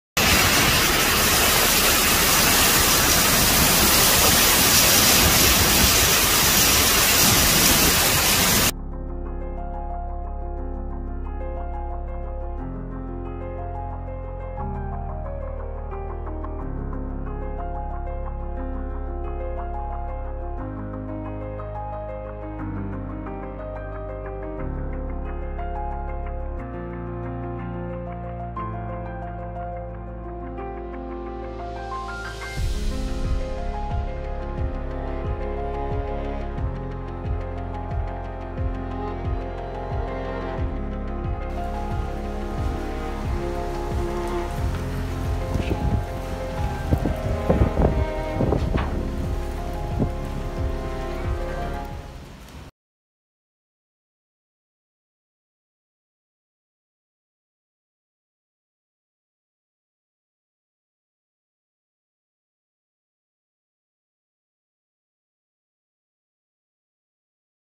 CTWANT 國際新聞 / 不畏軒嵐諾直撲！ 韓男海邊直播下秒被沖走
軒嵐諾颱風遠離台灣後北上，增強為超強颱直撲南韓。一名男子在颱風夜跑到海邊，直播軒嵐諾帶來的強風巨浪，沒想到下一秒一陣大浪襲來，他整個人被沖到雙向道中央。男子的褲子被海浪沖下，還意外露出股溝，他趕緊爬起，繼續高舉自拍棒，緊盯手機確認直播畫面。軒嵐諾威力達標，被南韓當局稱為「史上最強颱風」。6日凌晨4時50分登陸南韓，僅兩小時後便出海離開。